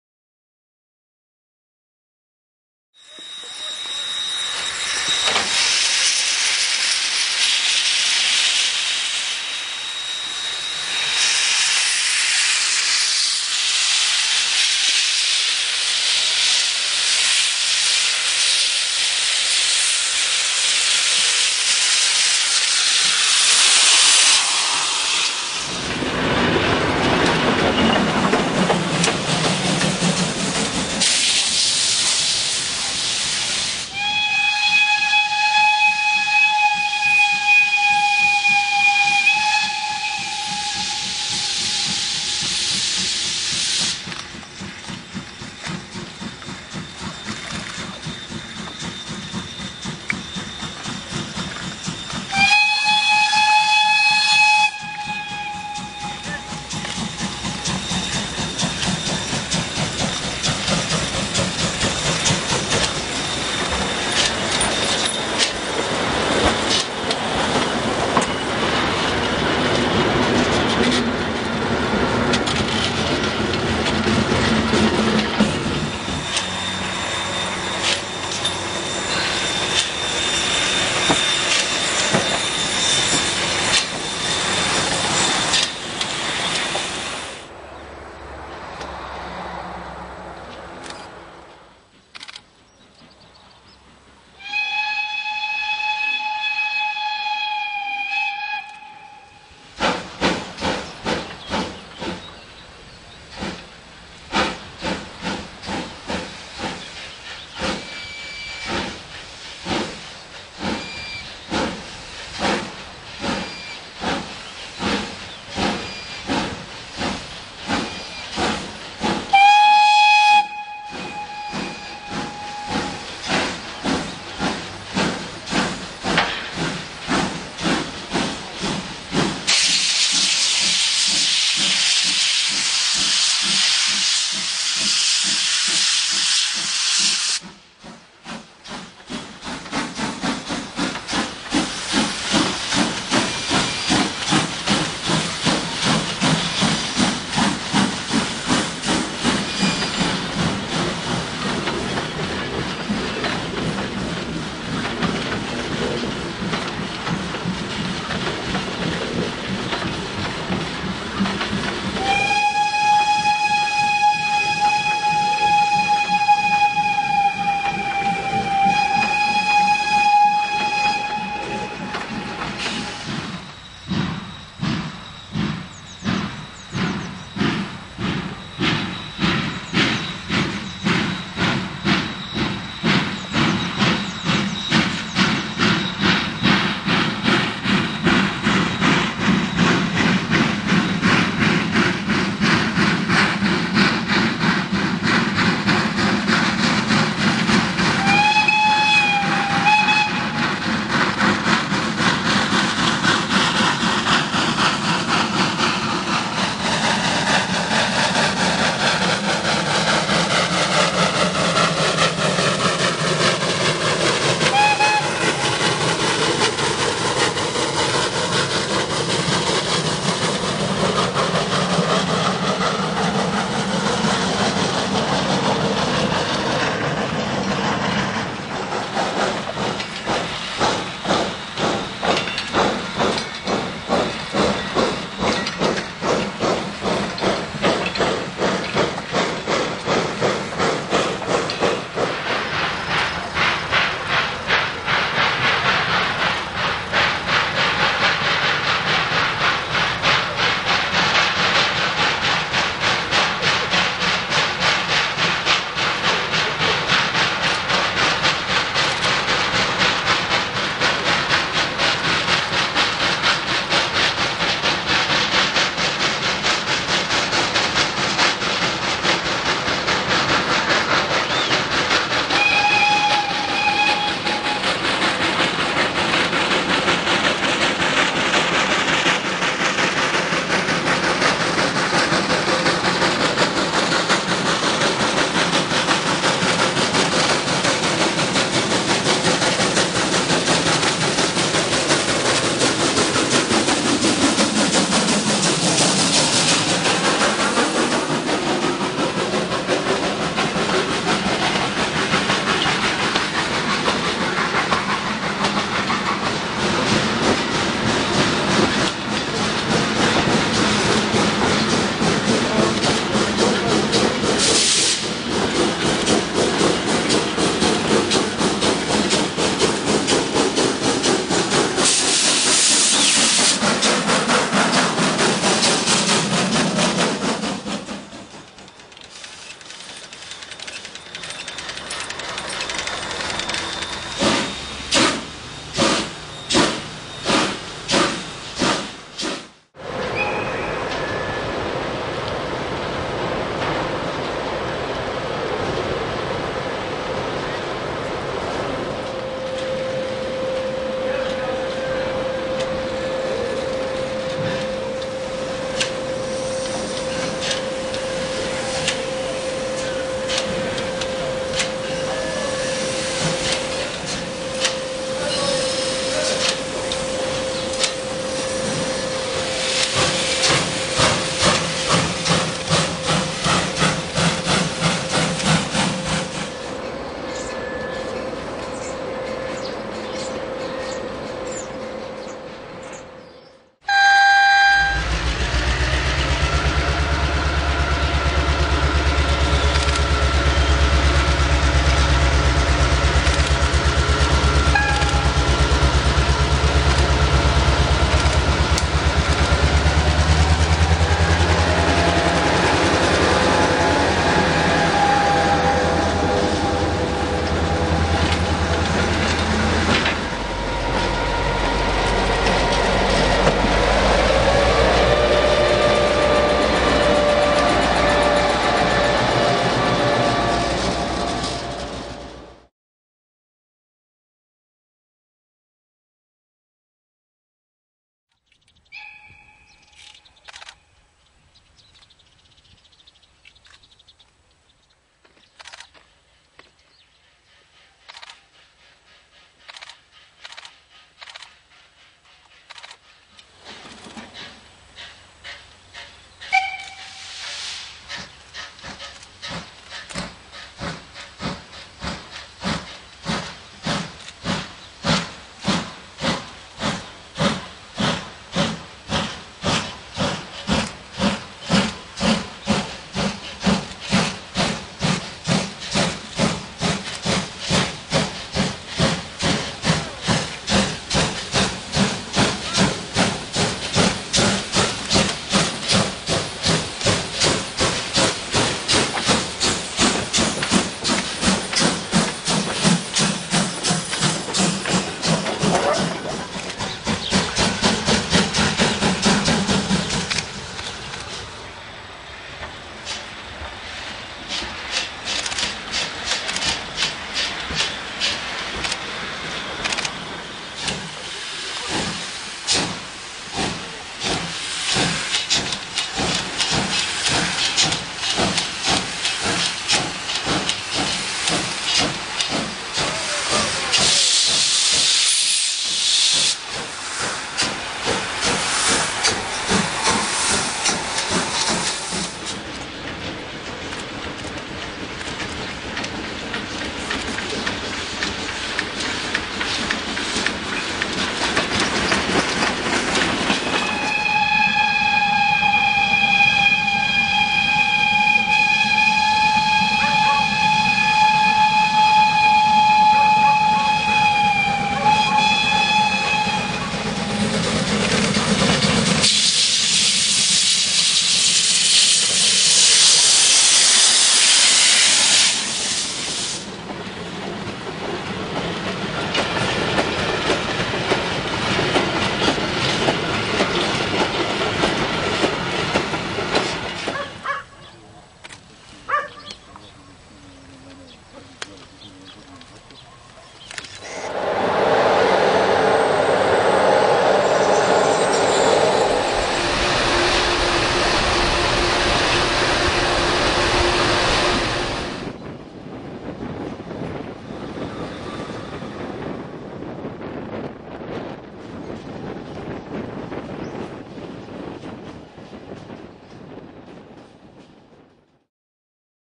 Bosnia Steam Tour 2010: 9 - Dubrave-Ljubace and ride home, April 6-9
33-236 (built as German 52 817) on duty at Dubrave mine and on the line to the ZFBH exchange station Ljubace, where class 661 GM diesels are taking over full coal wagons and leaving empties.